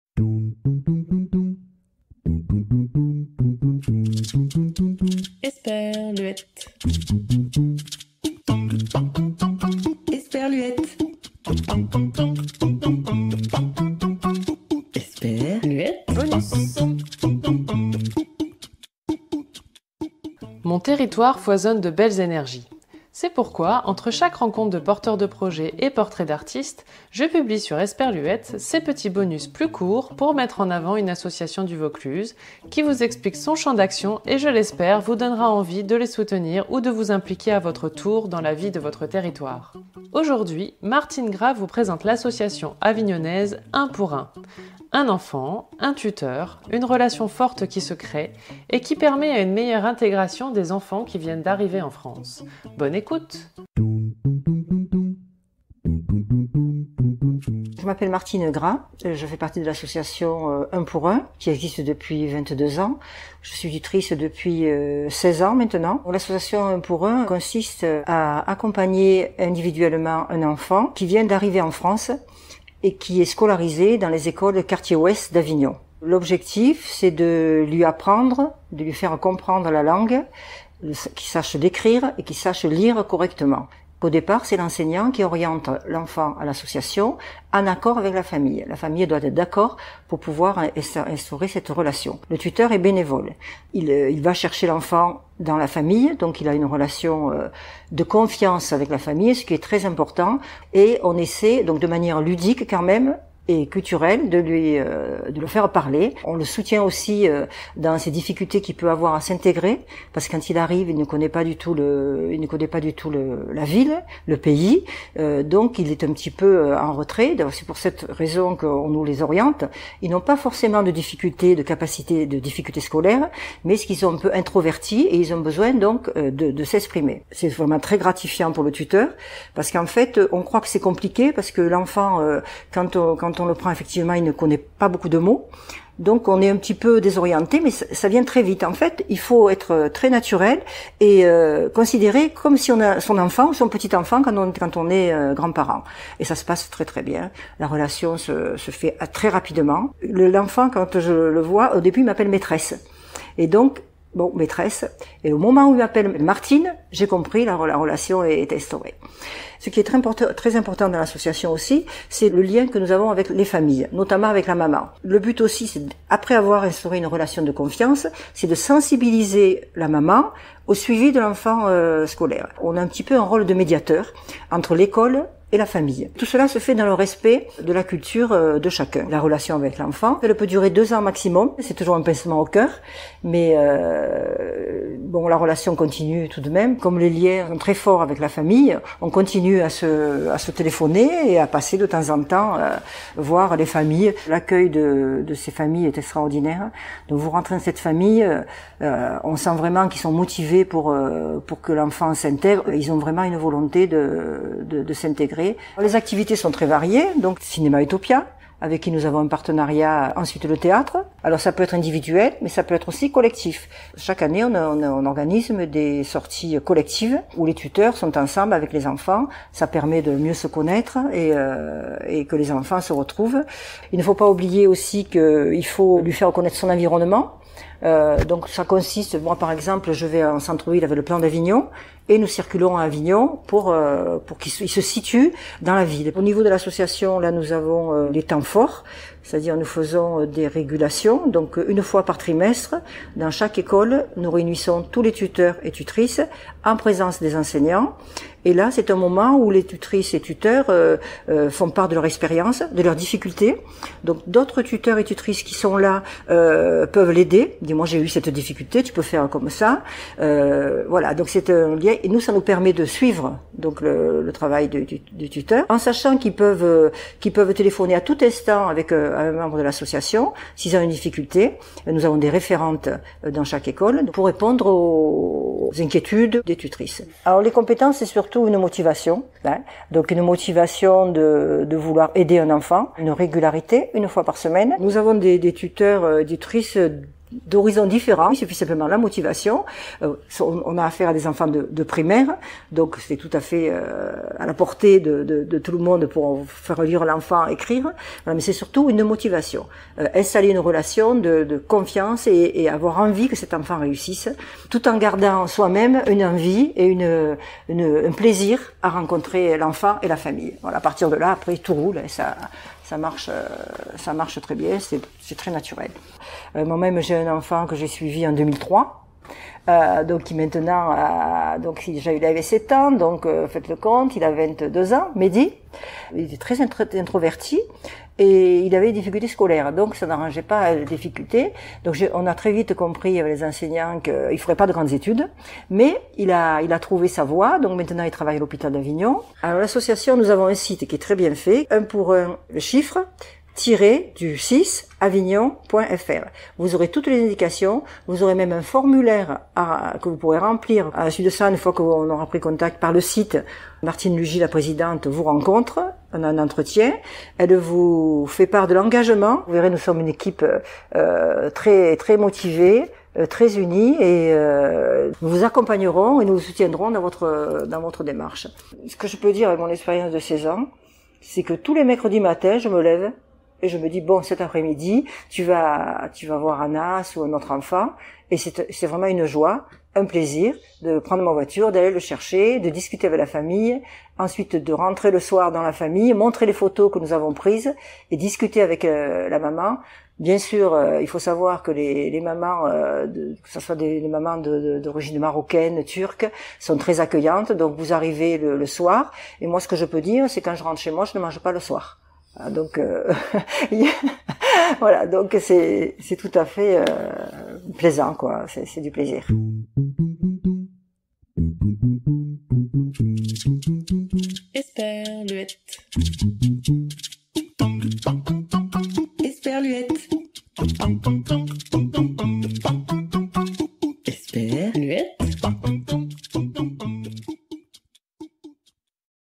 L'association Un pour Un - Esperluette Podcast (version sous-titrée)
Rencontre avec Martine Gras qui nous présente l'association avignonnaise Un pour Un.
Un enfant, un tuteur, une relation forte qui se crée et qui permet à une meilleure intégration des enfants qui viennent d'arriver en France.
L'association recherche en permanence des nouveaux tuteurs.

Pour plus de renseignements et tenter l'aventure, rendez-vous sur le et remplissez le formulaire ou prenez contact par téléphone.
J'ai moi-même été bénévole et ce fut l'une des expériences les plus enrichissantes que j'ai vécues, alors n'hésitez pas !

Esperluette est un podcast natif produit par Marie-Cécile Drécourt